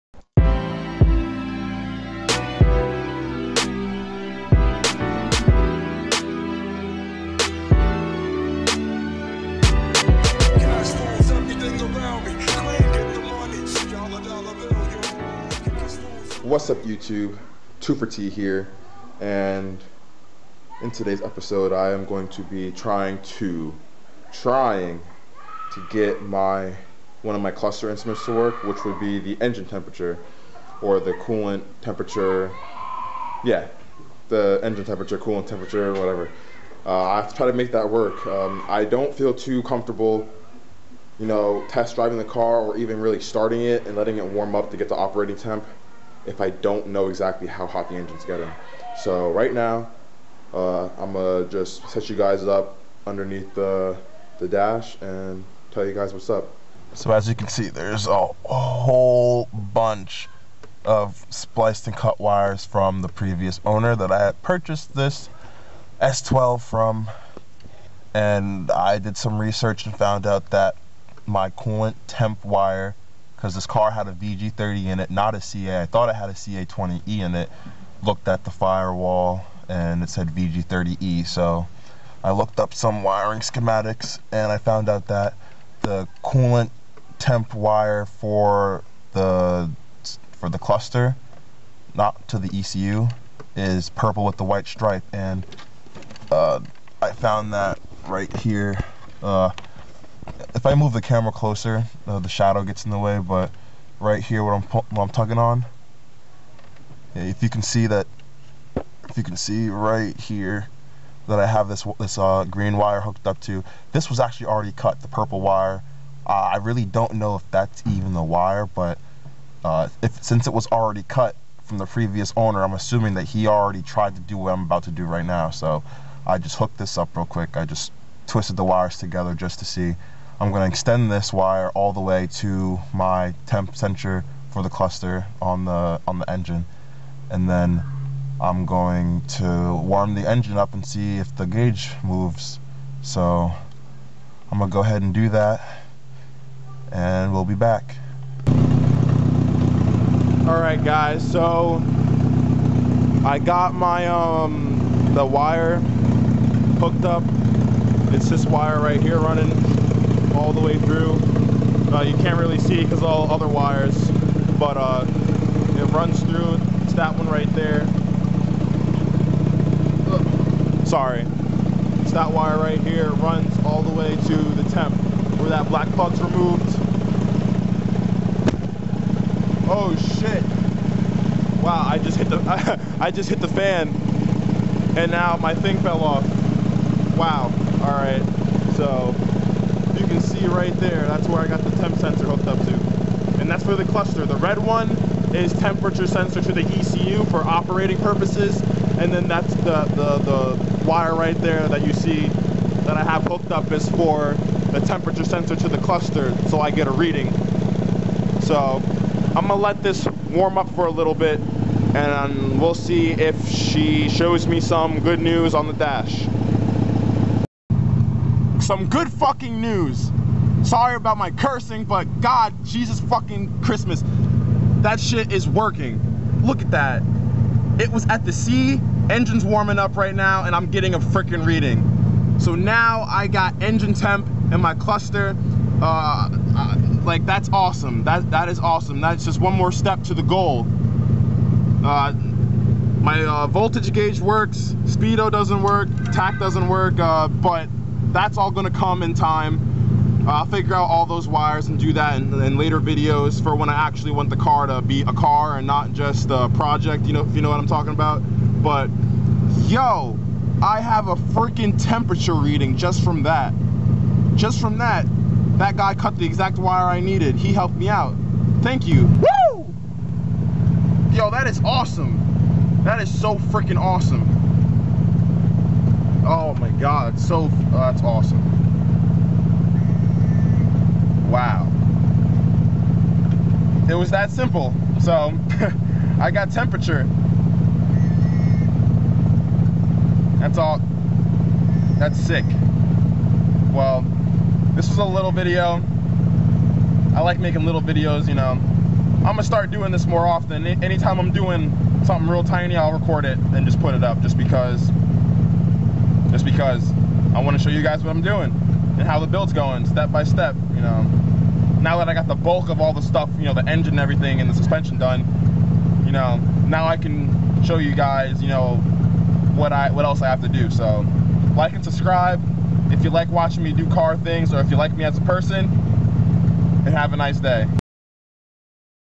S12 Prep Part 2: Wiring The Coolant Temp Gauge
I didn't feel comfortable starting the engine anymore without knowing the engine temperature so I did some research on how to wire my ka24de temp sensor into my s12 cluster.